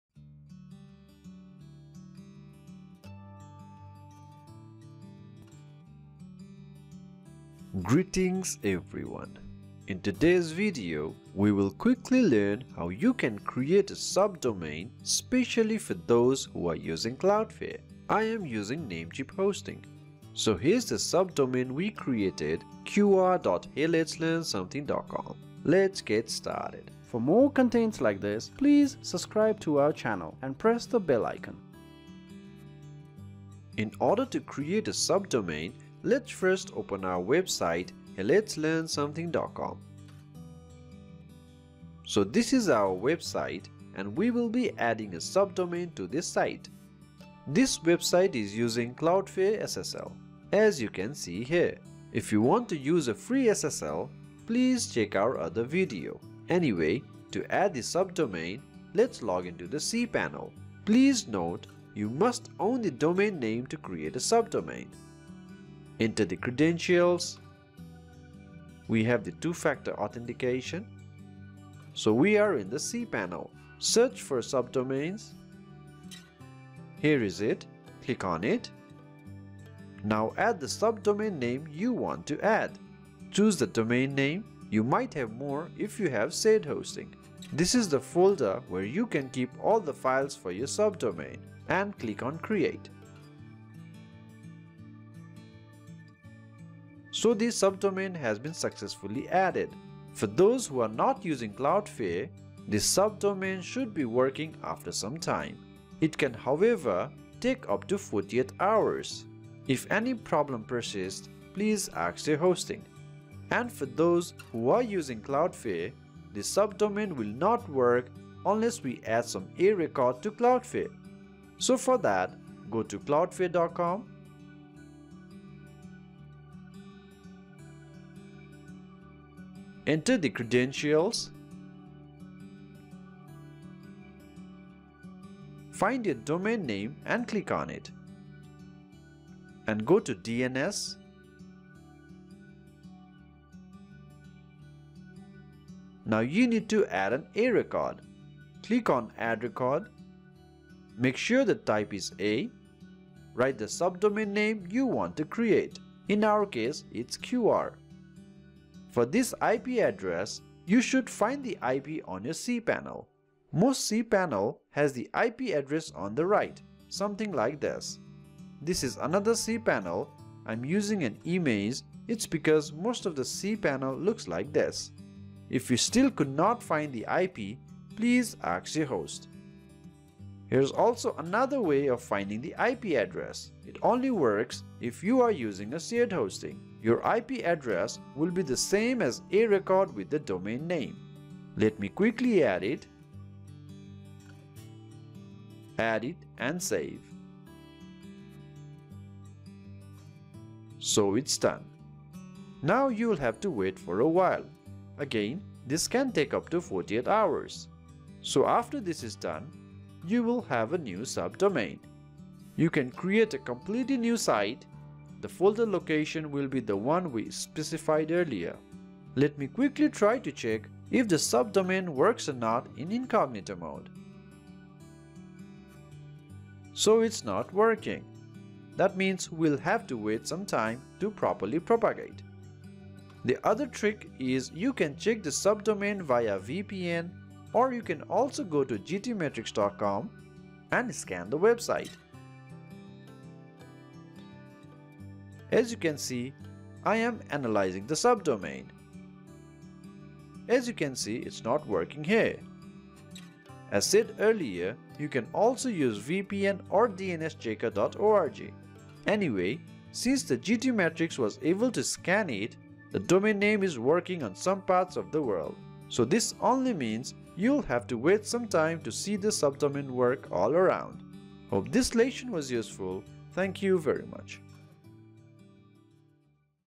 How to create a subdomain using a cPanel, guide to Cloudflare DNS subdomain? WordPress 2022
WordPress subdomain: In this video, you will quickly learn how you can create a subdomain for your domain name especially for those who are using Cloudflare SSL.

After creating the subdomain you can easily install WordPress or also use other PHP or HTML codes to create a completely new website. The subdomain will act similar to a new domain name so you can assign a specific folder to it and work as you want.

Those who are using Cloudflare SSL will have to add an A record in order to make the subdomain work. The whole process has been clearly explained in the video.

Hope you learned something interesting!

We got 95+ scores on Google PageSpeed Insights for heyletslearnsomething.com!